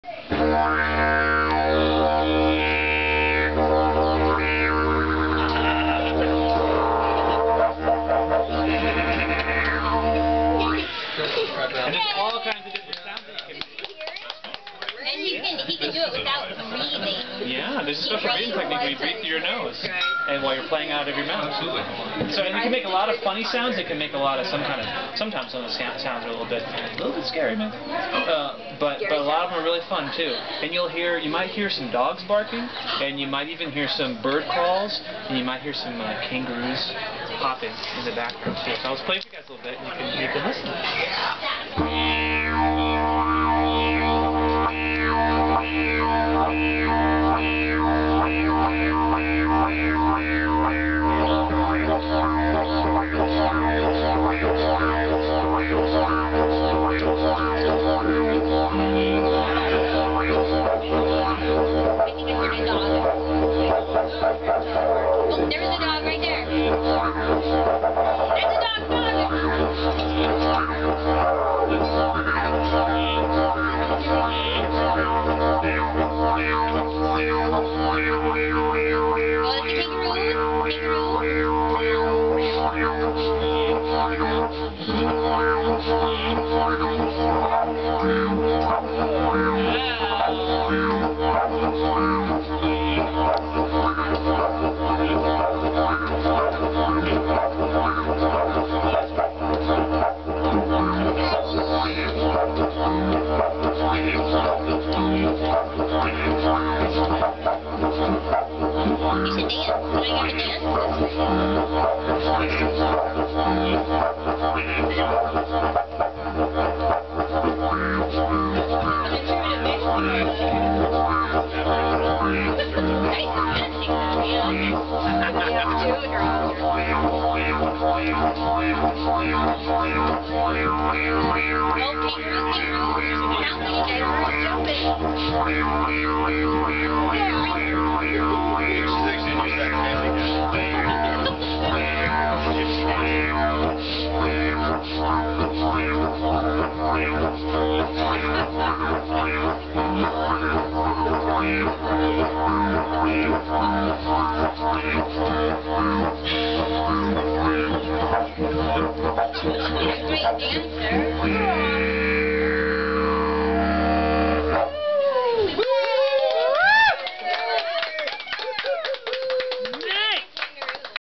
Didgeridoo by Nathan Michalski at the wedding of Cat and Ben Walker